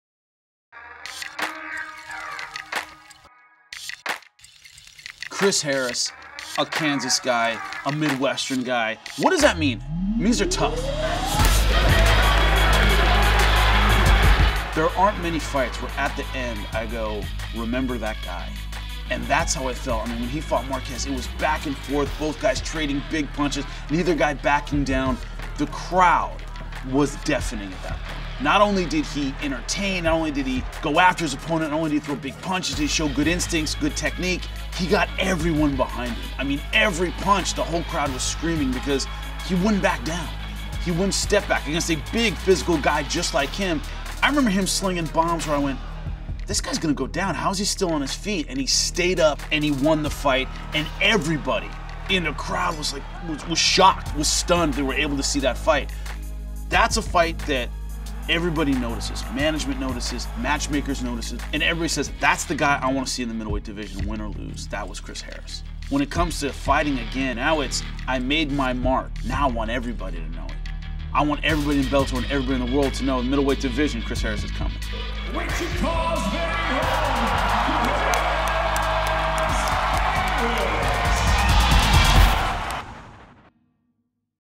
Bellator 171: Foundations | Chris Harris
Chris Harris fights once again in front of a hometown crowd! Can he bring out the scrappy Kansas style once again??